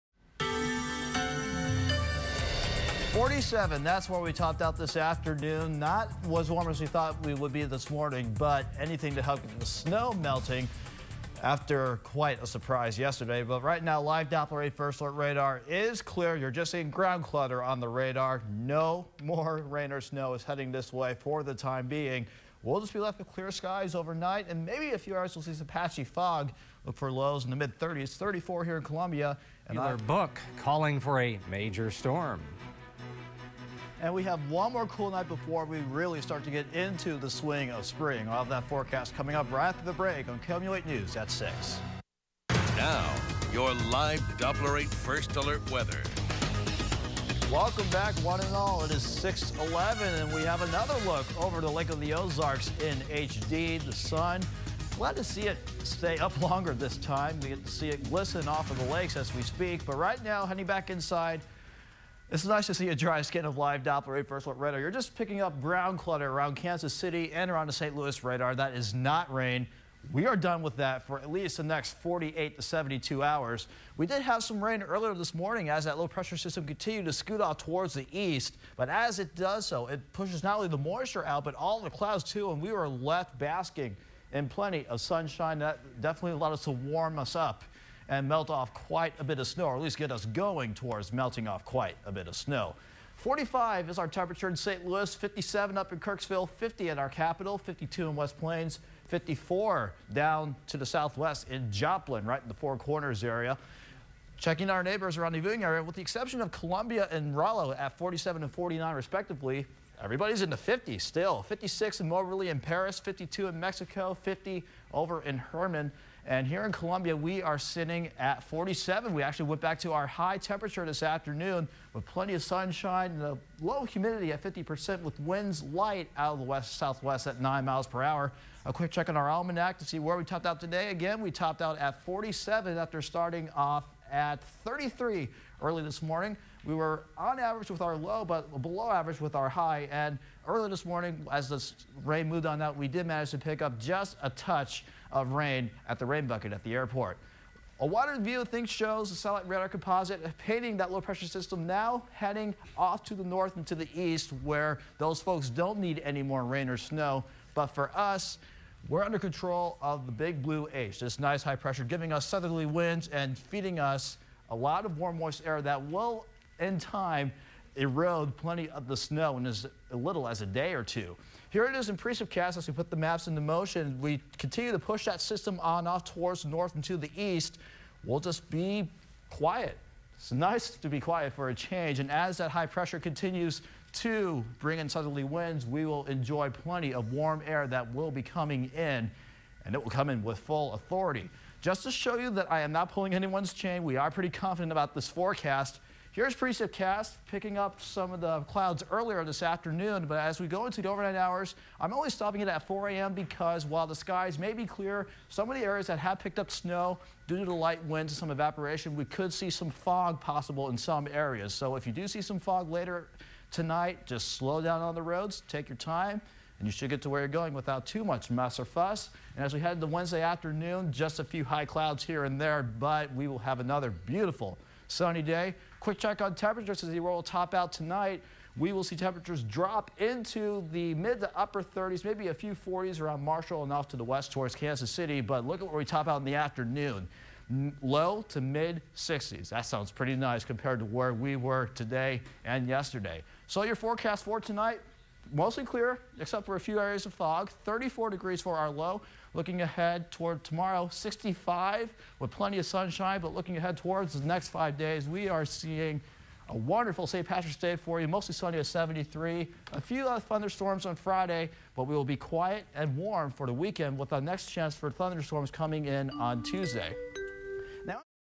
March 15th 6 PM Weather
My forecast for Mid-Missouri on March 15th during the 6 PM Newscast and the day after everyone said there would be no snow on the previous Monday...and we ended up seeing snow around Columbia.